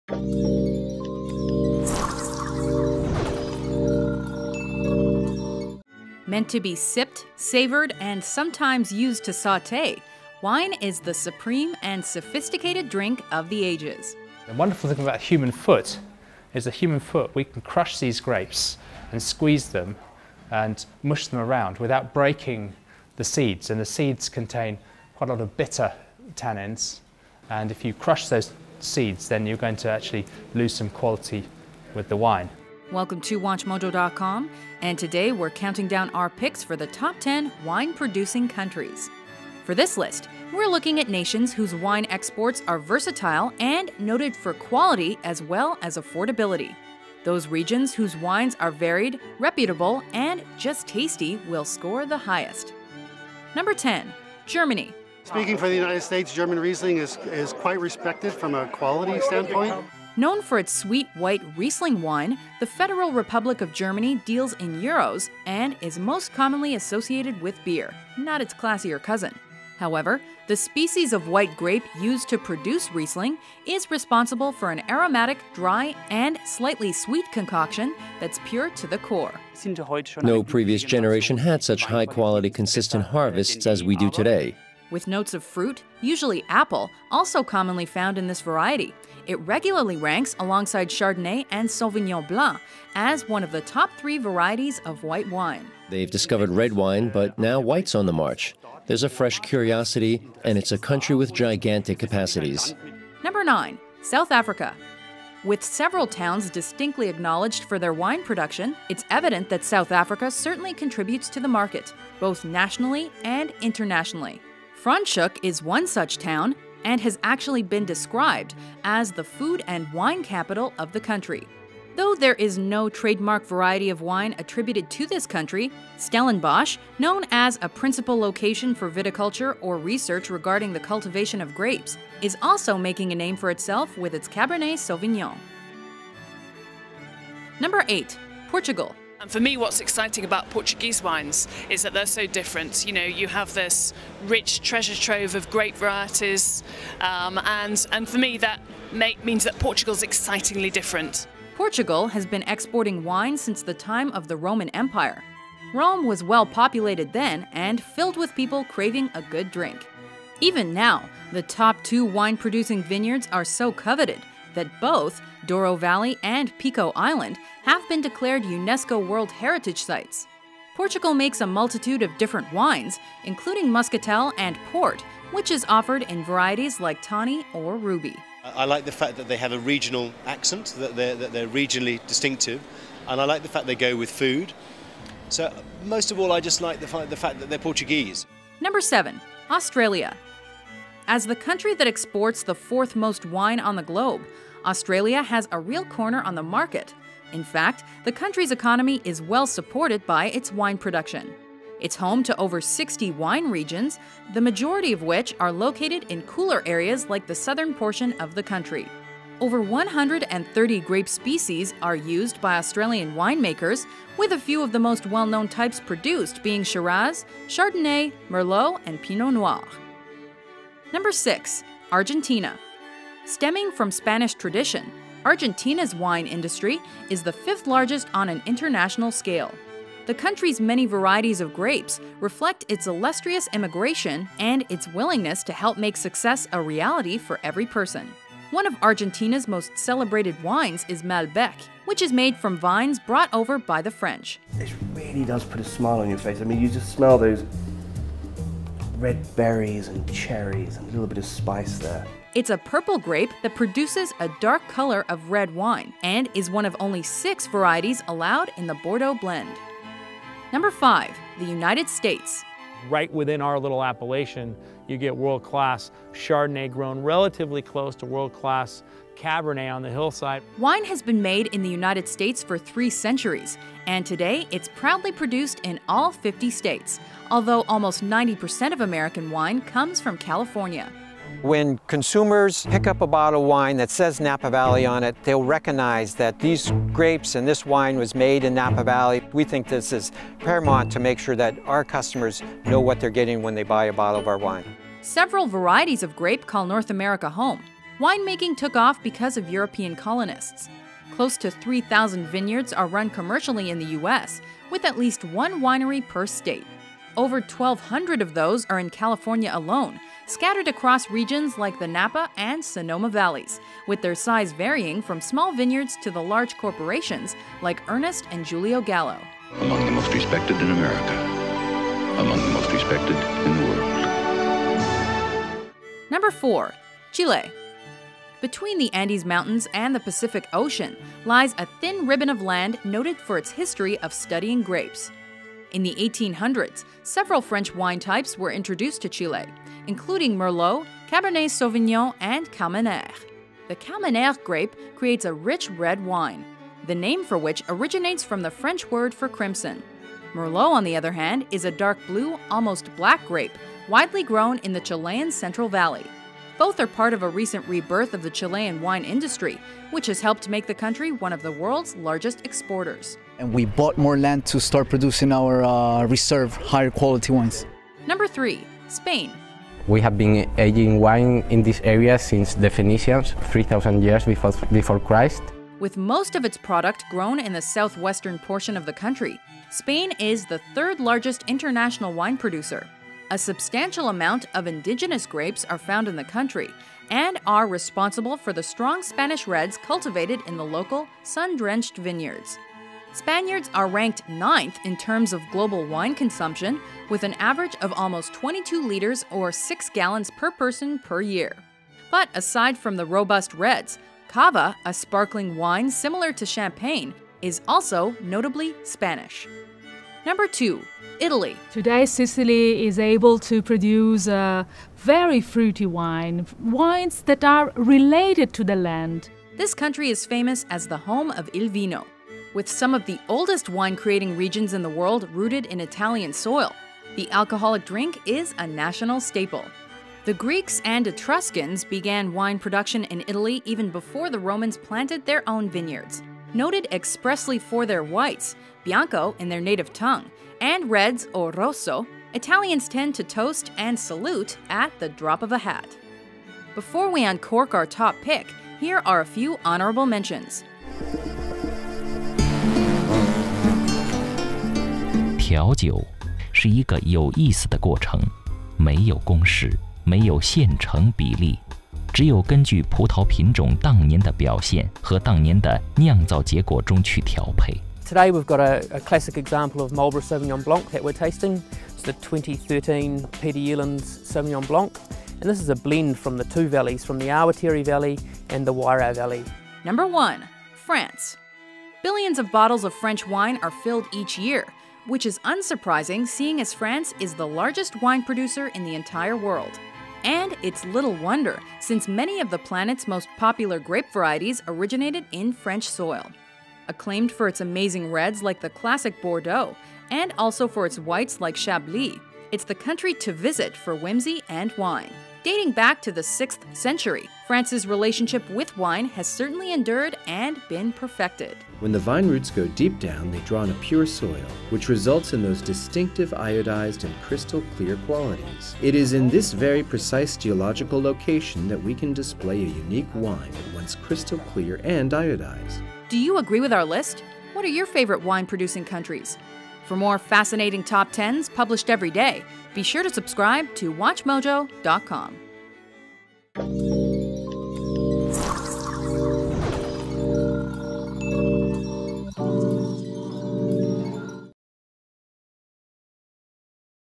Top 10 Wine Producing Countries
Meant to be sipped, savored and sometimes used to sauté, wine is the supreme and sophisticated drink of the ages. as we count down our picks for the top 10 wine-producing countries.

Special thanks to our users akt, Oakley.24 and akaki makacaia for submitting the idea via our Suggest Tool